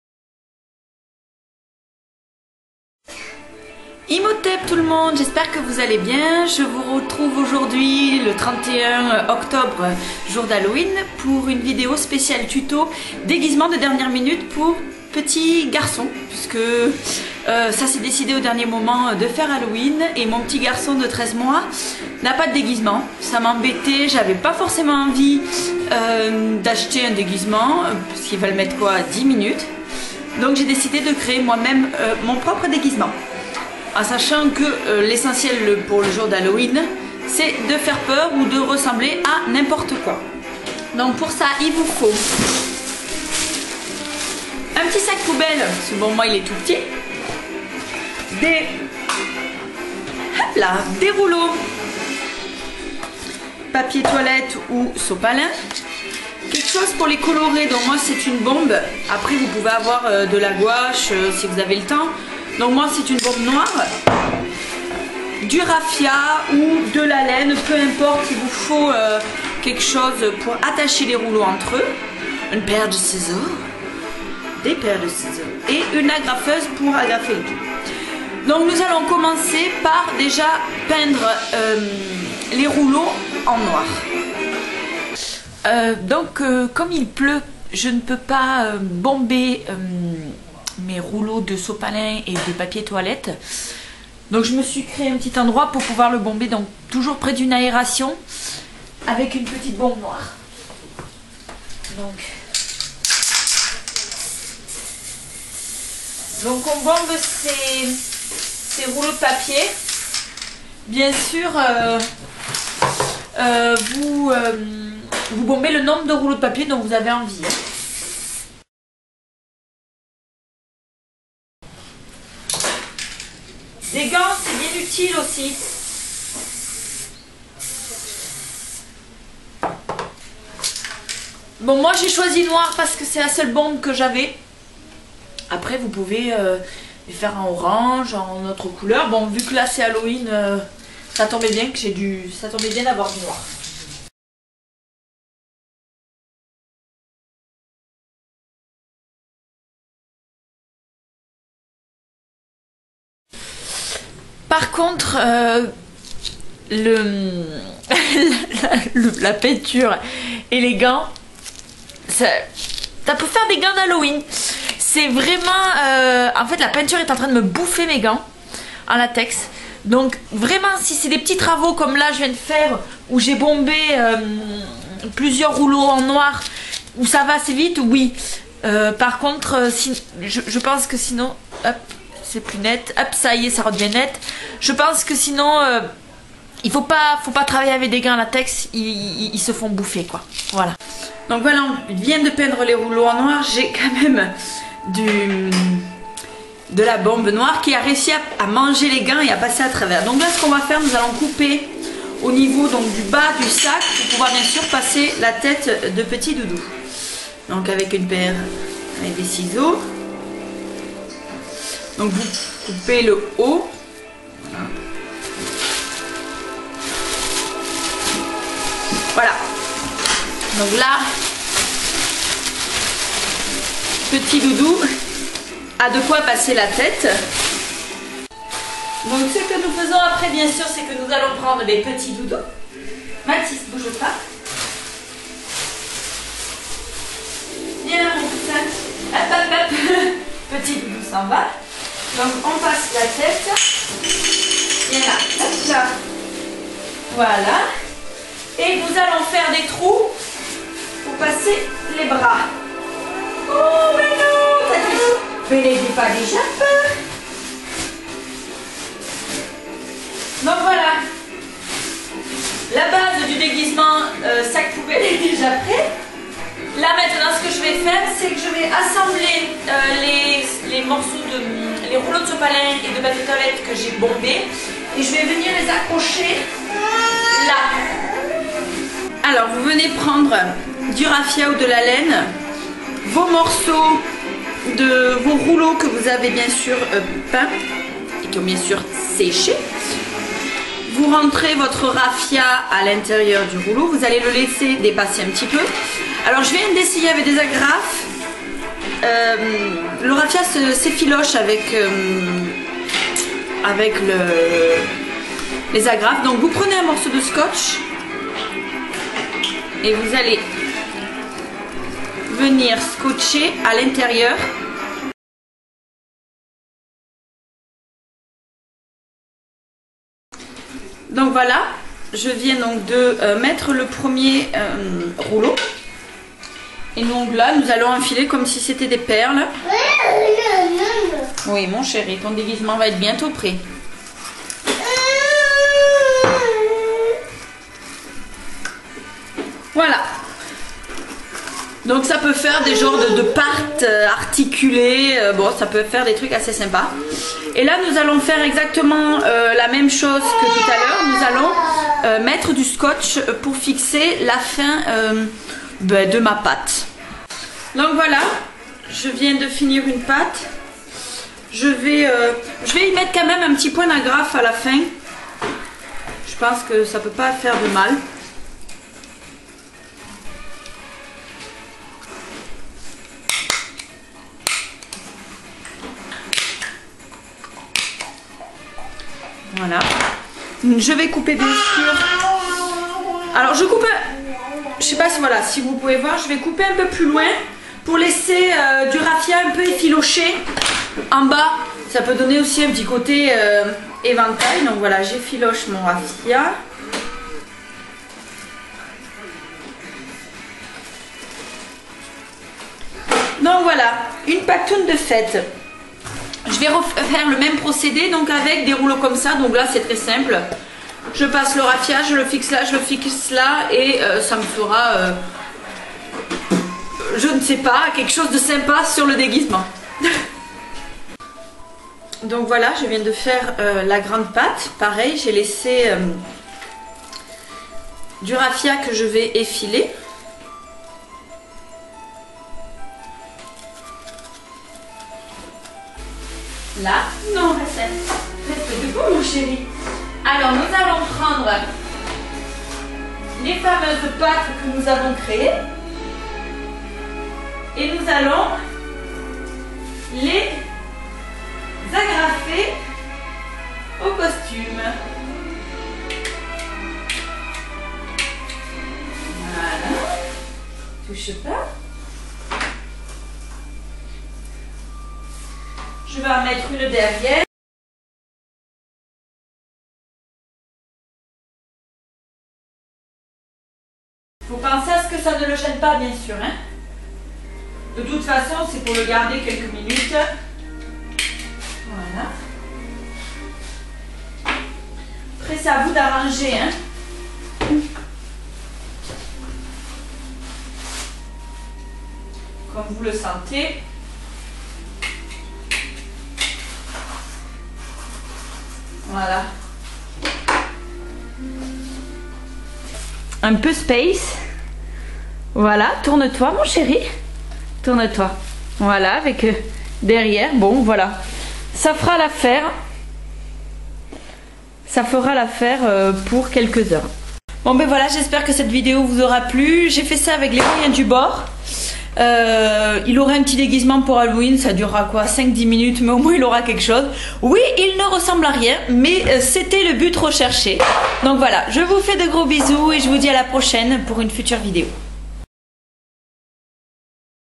Lady Marjo tuto déguisement d'Halloween fait main.
Un petit tuto pour réaliser un déguisement d'Halloween qui ressemble à rien ou a une poubelle peut-être!!! Mais le but c'est d'être déguisé pour le soir d'Halloween .
Avec la participation de mon doudou.